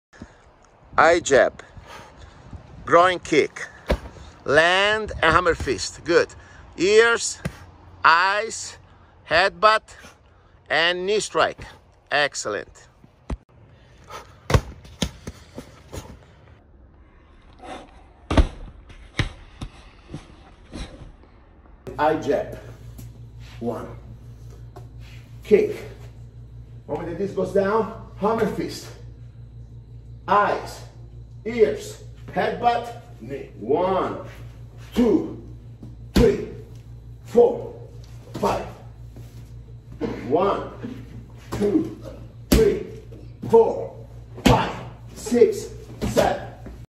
Krav-Maga 7 Strikes Drill
Eye Jab
Groin Kick
Hammer Fist
Ears slap
Eyes gouge
Head Butt Strike
Knee Strike